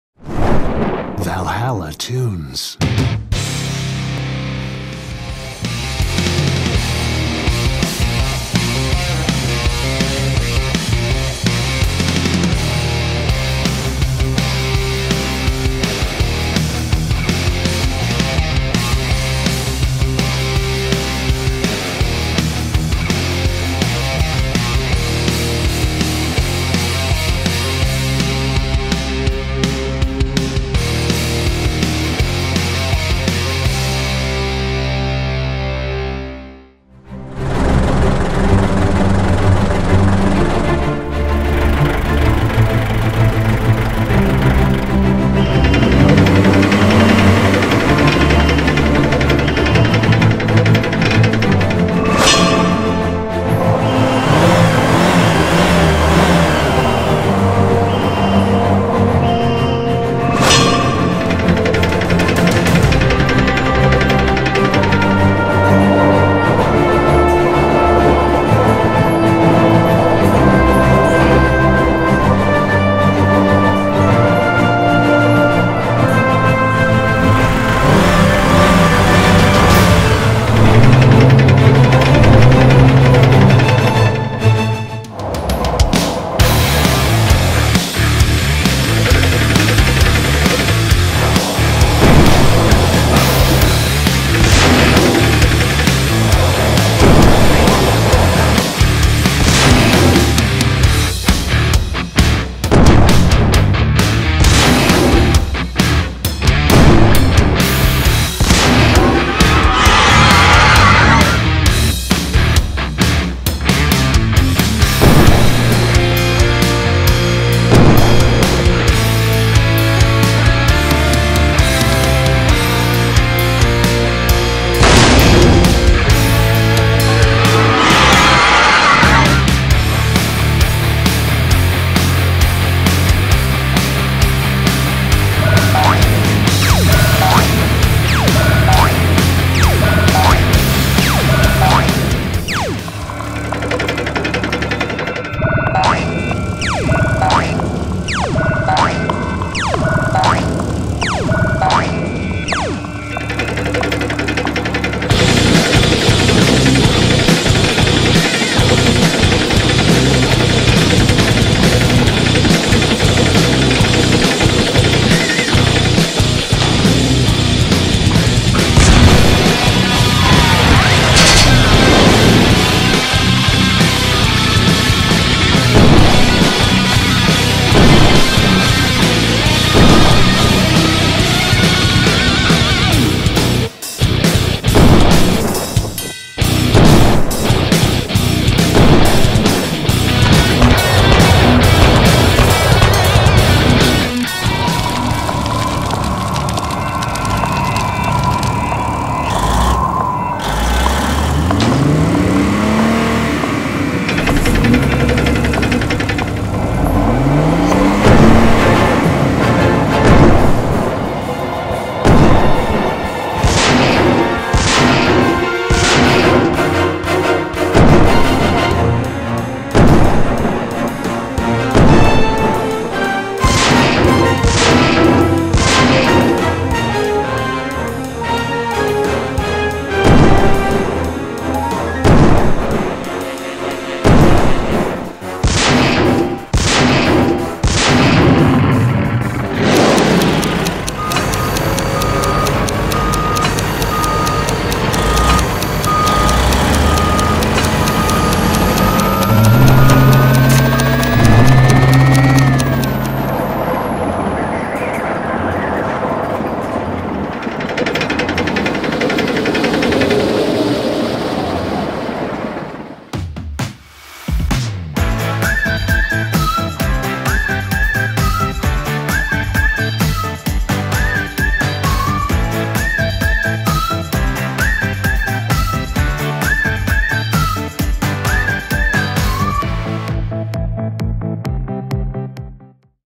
Tank Tournament Cocazilla VS Biotech - Cartoons about tanks
In this cartoon you will see a new episode of the Tank Tournament project. This time, two over-powerful iron monsters will meet each other on the battlefield. A steel giant named Biotech will fight against the monster Cocazilla. By the way, this steel giant is based on the well-known Dorzilla tank. Well, one way or another, but both rivals have impressive capabilities and therefore it is impossible to predict who will win this tank duel. In general, if you have already seen the video from the Tank Tournit series and you liked those videos, then I hope that this new cartoon will also attract your attention.